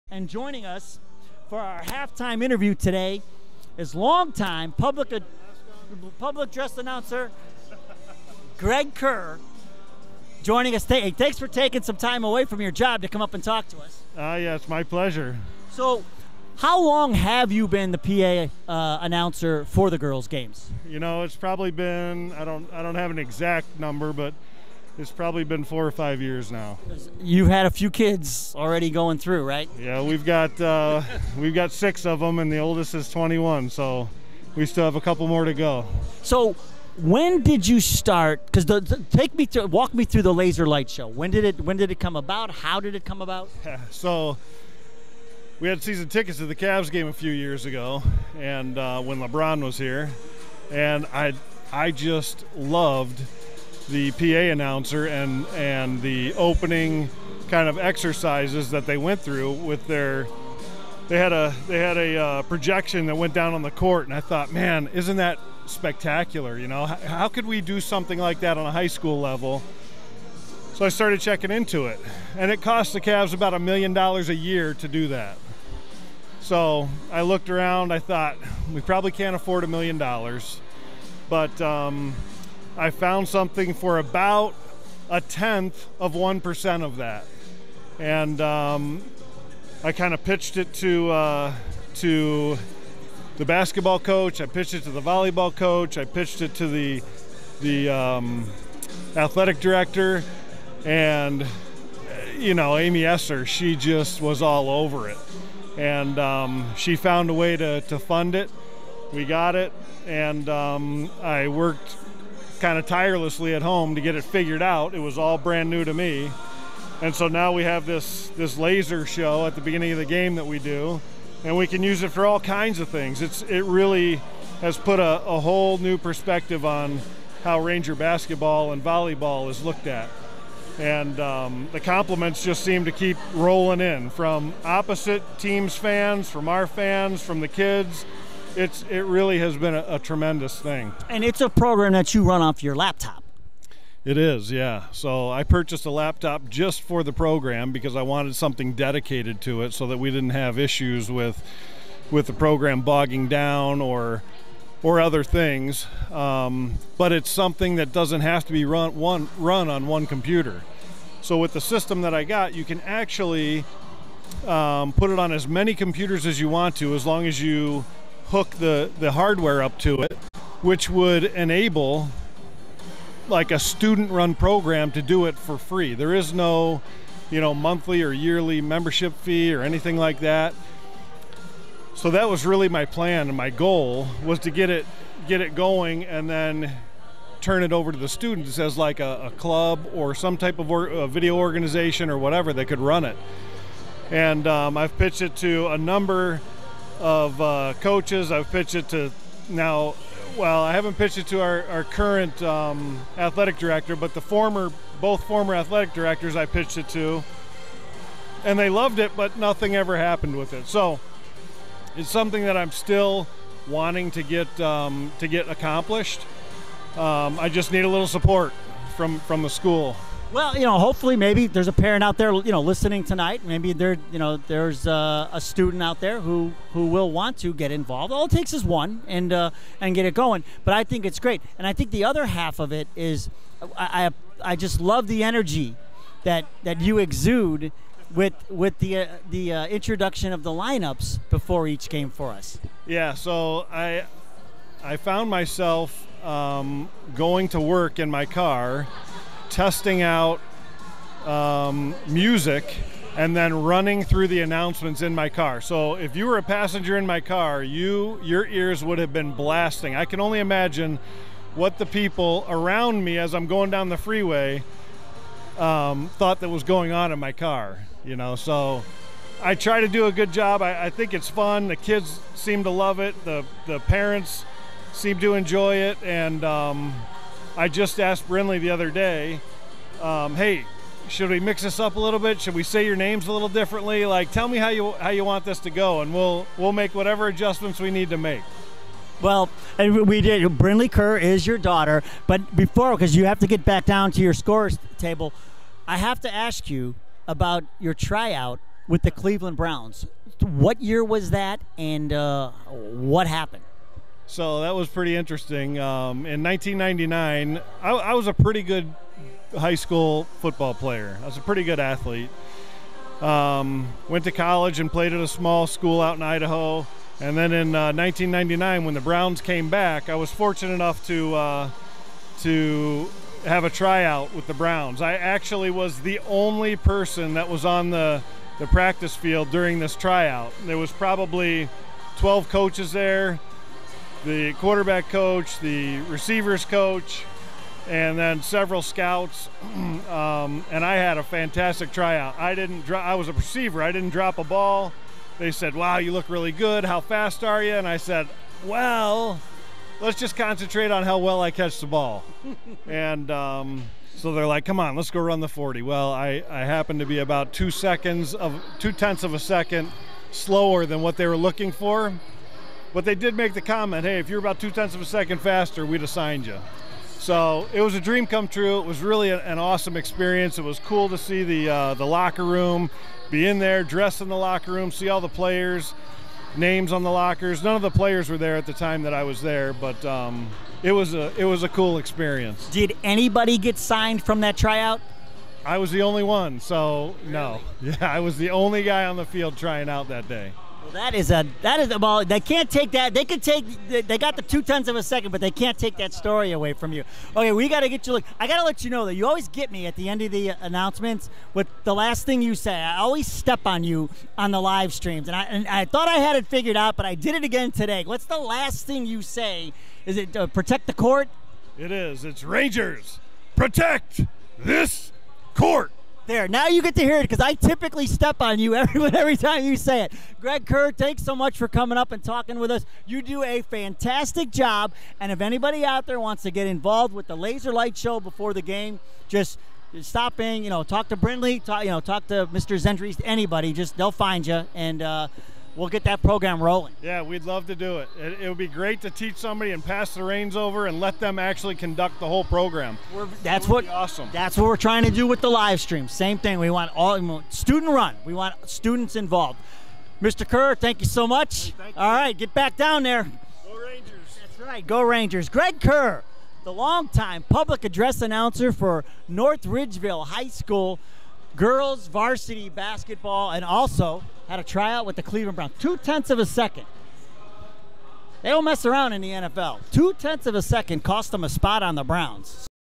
Greg Kerr Interview: Pregame Shows and Cleveland Browns Tryout
He's the mastermind behind the laser-light show you see, and the booming voice you hear before & during every @North Ridgeville High School Girls basketball game.

During my halftime interview with Public Address announcer Greg Kerr, we talk about THAT and his tryout with the Cleveland Browns (back in the day). A great sports interview filled with a lot of great sports interview questions.

He takes the girls' high school basketball pregame show to another level.